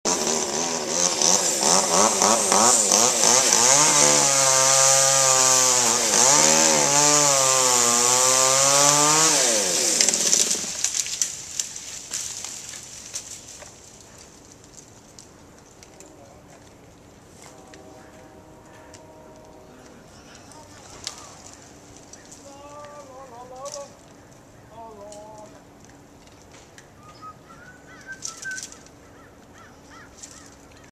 Tree Fall-Awesome.mov
Sometime's it's just cool to see big stuff fall. Check out a tree trimmer on DiggersLIst as they cut off one of the top branches. TIMBER!!!